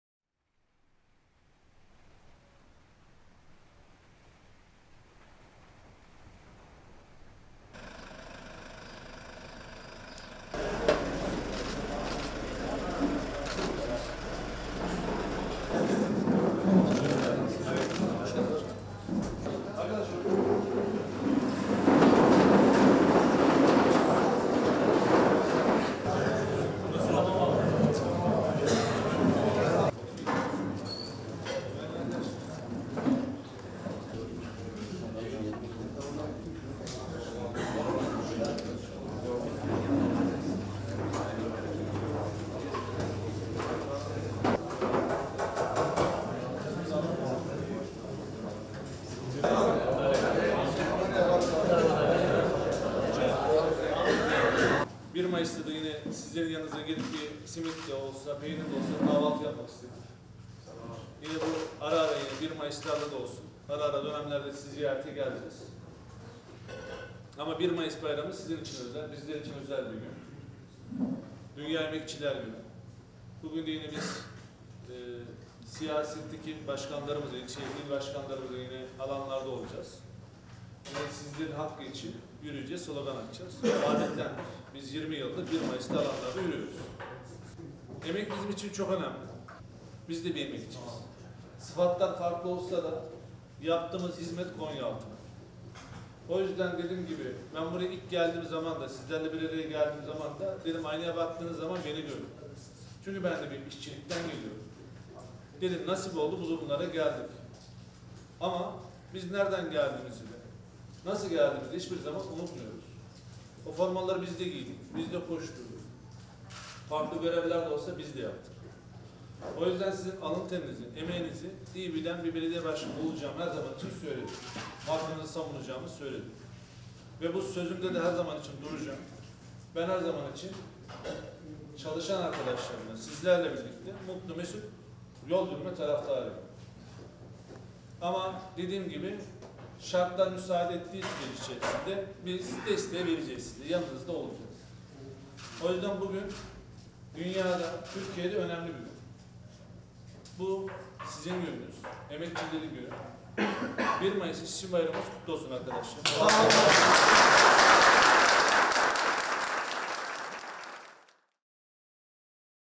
Başkan Kotan, işçi bayramı sabahında güne işçilerle başladı
Konyaaltı Belediye Başkanı Cem Kotan 1 Mayıs Emek ve Dayanışma Günü’nde sahada mesaide olan personel ile kahvaltıda bir araya geldi. Başkan Kotan, işçilerin her zaman yanında olacağını ve haklarını savunacağını söyledi.

Montaj&Kurgu: Uğur FİDAN